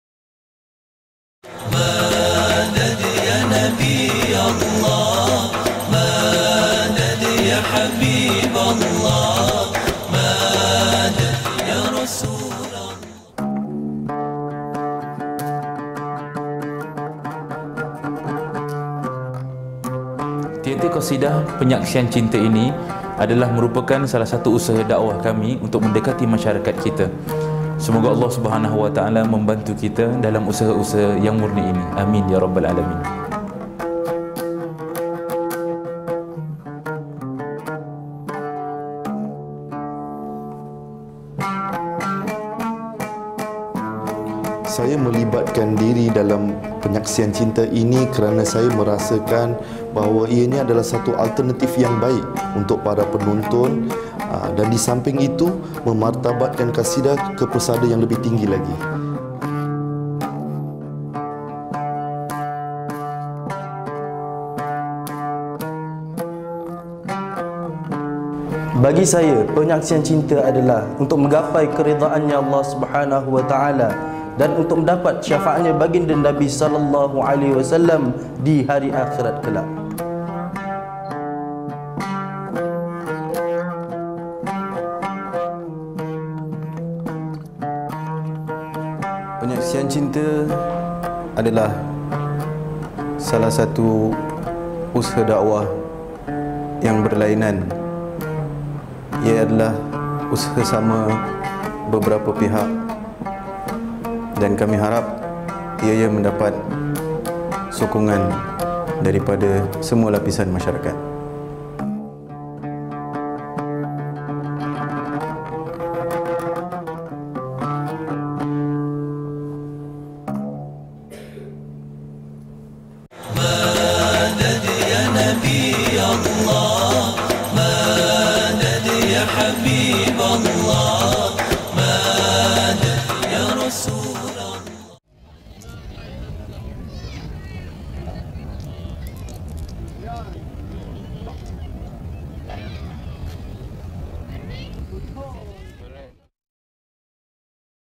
"Penyaksian Cinta" (Official Teaser II) - Akhina Yusri Ibrahim (Al-Khidmah)
Teater Qasidah : "Penyaksian Cinta" project:

is our first ever major project in trying to deliver a Dakwah by gathering talented individuals that are willing to get together and working hard for a single common cause, delivering the message of Tauhid and the Vow of Love towards our Creator and our prophet.

We never expected it to be easy especially with the high costs involved and the very very few commercial sponsor that are willing to come forward to provide us with sponsorship. Thus, we are relying a lot on individual sponsors that bought the VIP package tickets.
It's also not easy trying to make the cost reasonable enough so that people can come to the event as a family, an event that will be engrave into their memory as one of the many beautiful moments in their life.

Ultimately, it's the message of Allah's love towards his creations and Rasulullah s.a.w caring and concern for us in this world and the hereafter that means the most to us.